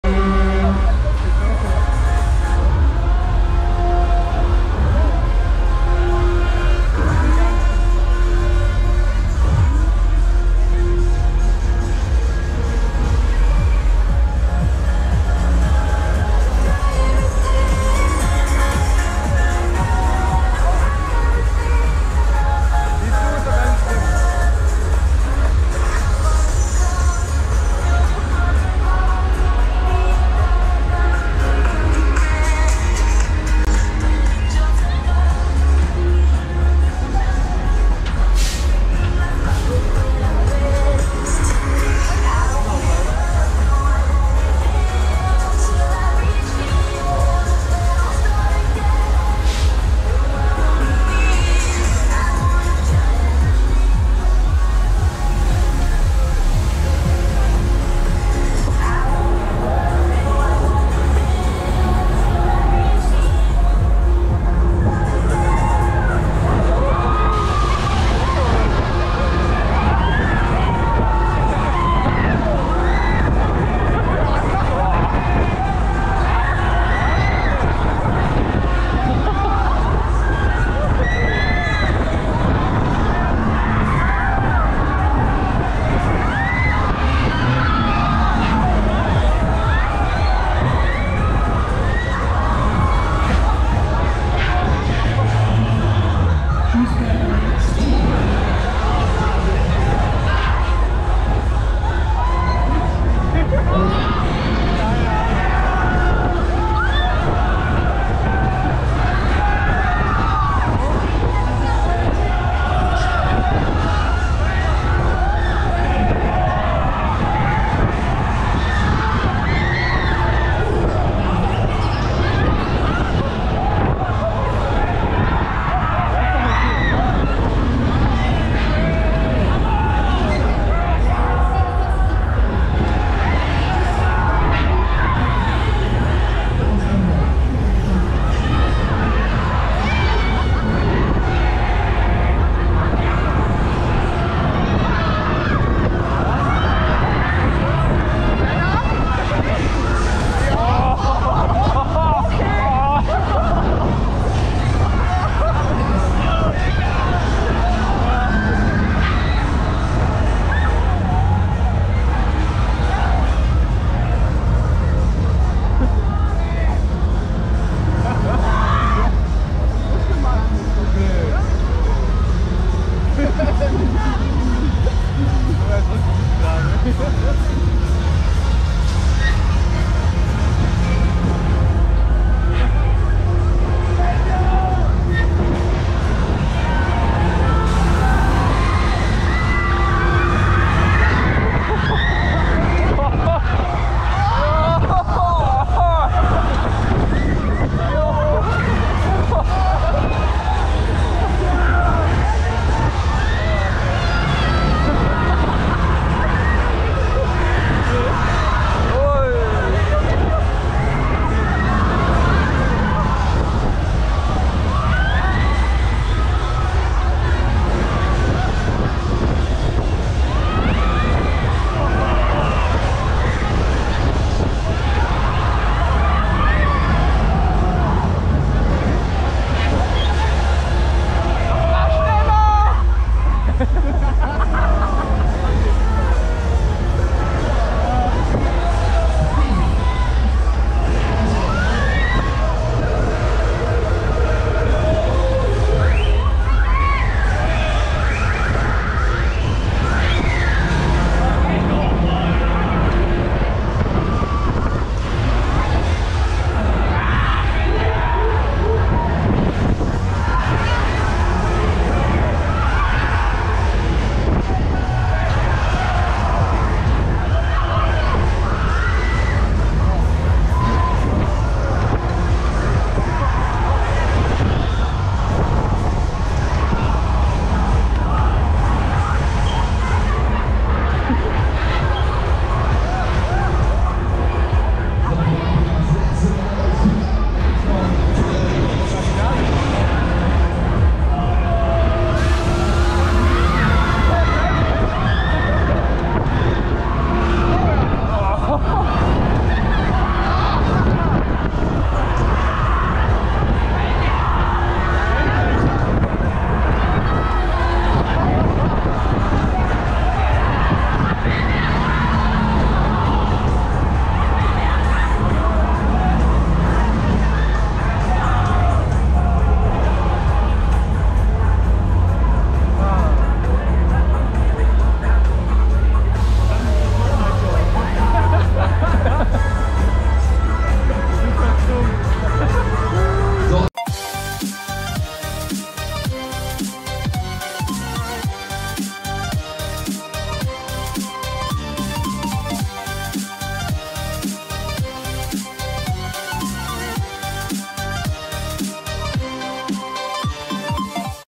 Steamer [Langhoff] | Steinert Kirmes Lüdenscheid 2023 | OnRide POV
Steamer - Steinert Kirmes Lüdenscheid 2023

Statistiken:
-Veranstaltung : Steinert Kirmes Lüdenscheid
-Hersteller : Huss
-Typ : Flipper
-Schausteller : Langhoff